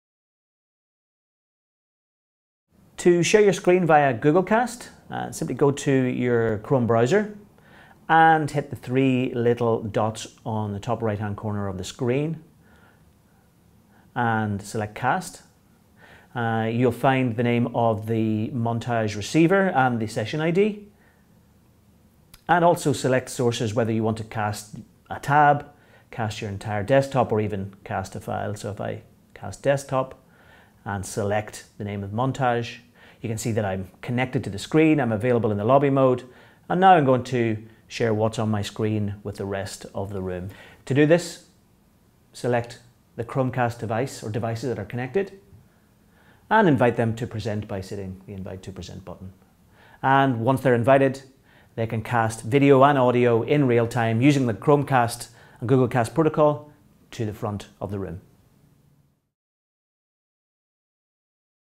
Share your screen using GoogleCast - DisplayNote Montage
Ed Morgan from DisplayNote shows us how to share your screen using DisplayNote Montage software.